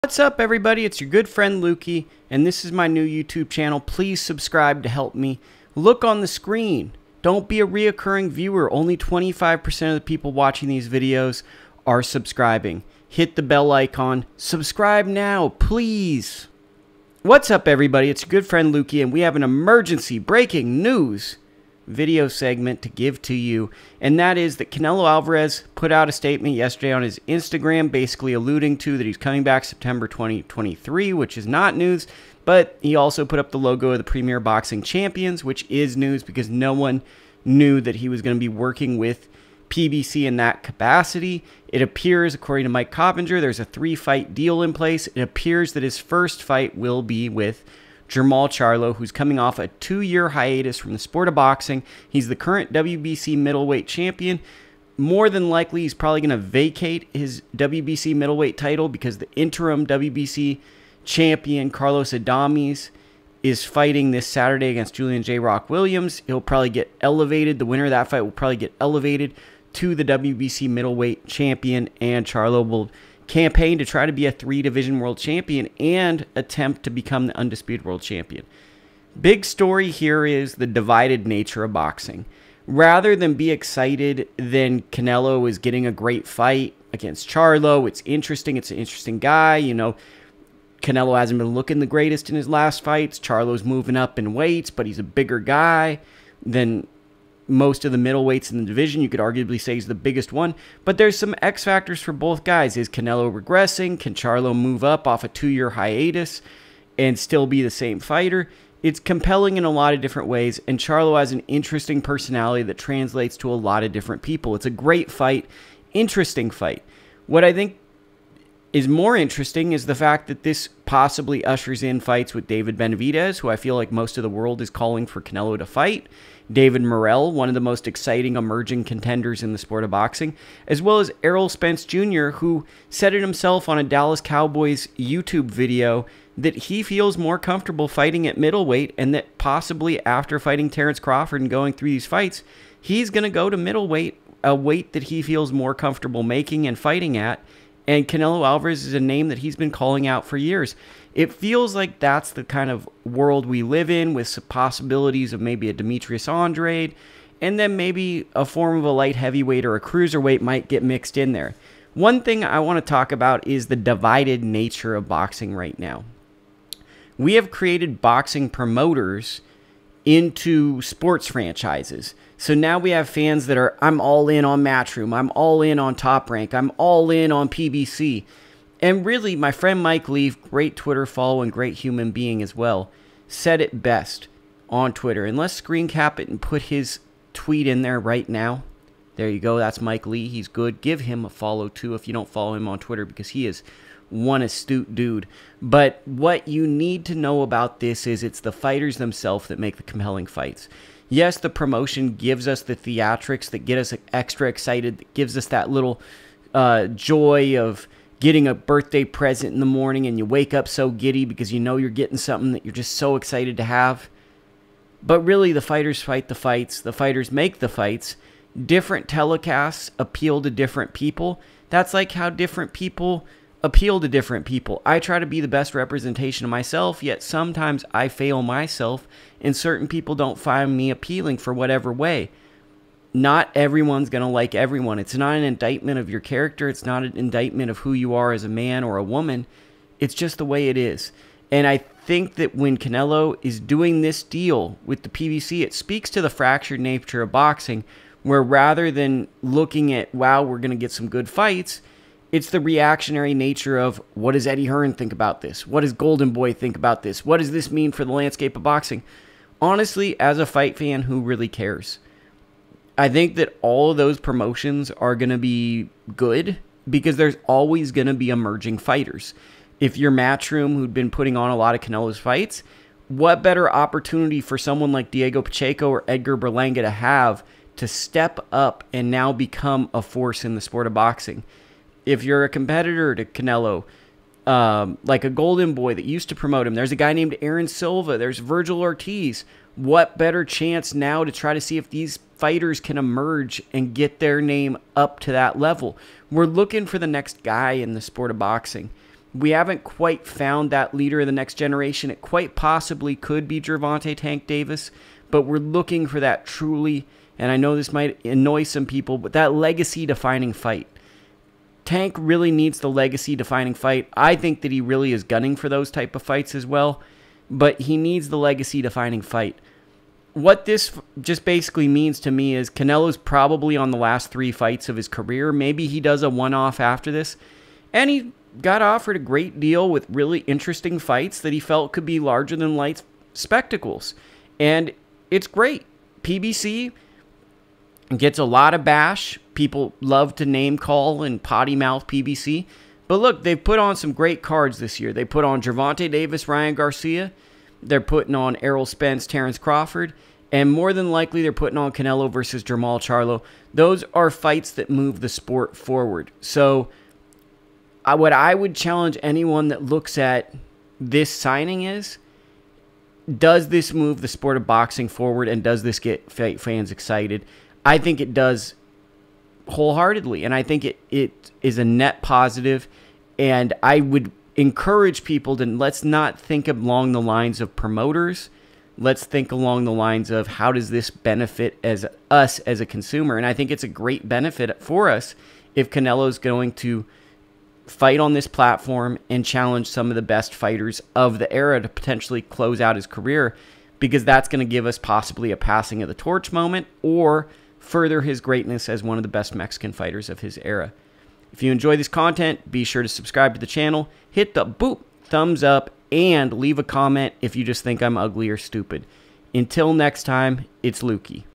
Canelo to PBC? Thoughts on a possible three-fight Premier Boxing Champions deal
Thoughts on Canelo Alvarez seemingly signing with Premier Boxing Champions for a three-fight deal, which may just be the end of his career.

Right now the rumored fights are Jermall Charlo next, with hopes of a future fight with either David Benavidez, David Morrell, and/or Errol Spence Jr.